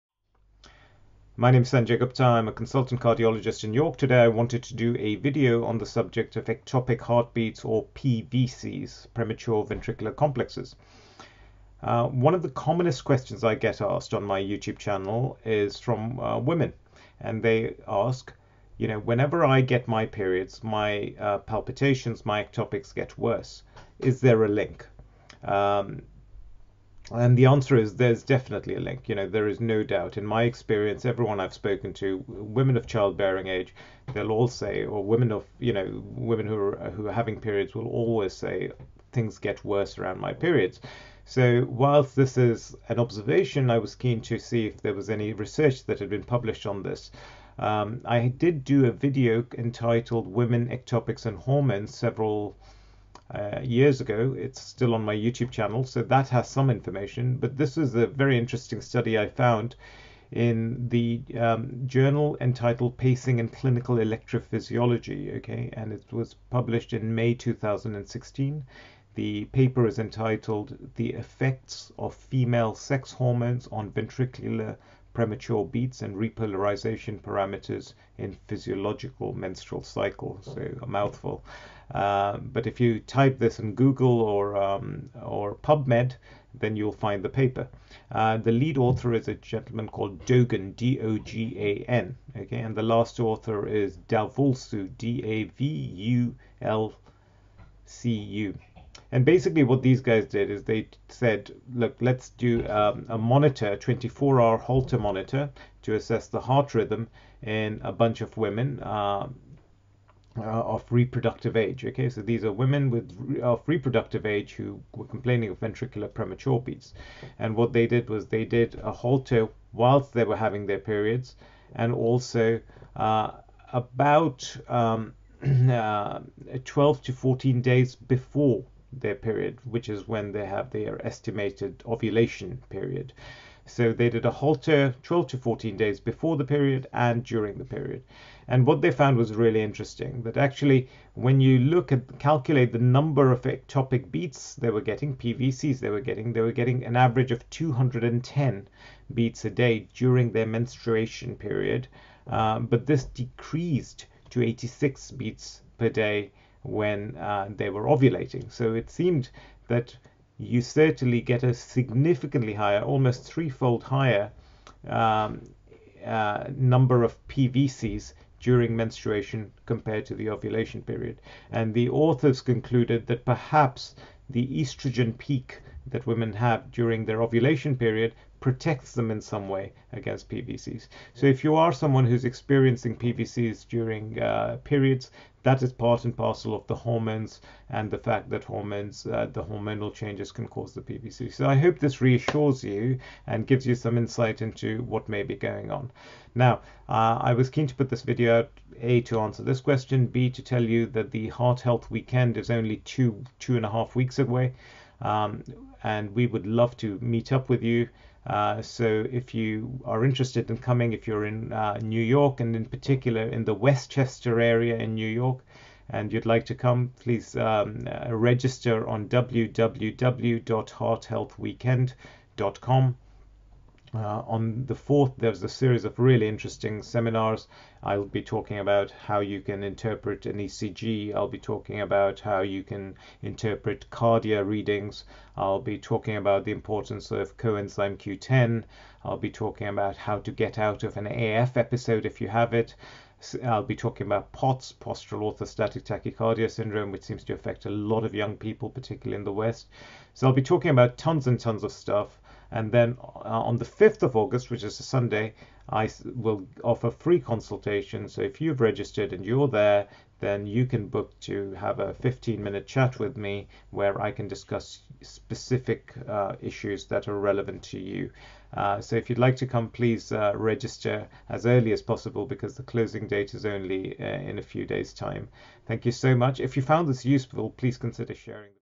Can hormonal changes make my PVCs worse
In this video, i discuss an interesting study which confirms the link between hormonal changes in women and increased PVCs which can be felt as heart palpitations. Please visi for a free 1 on 1 consultation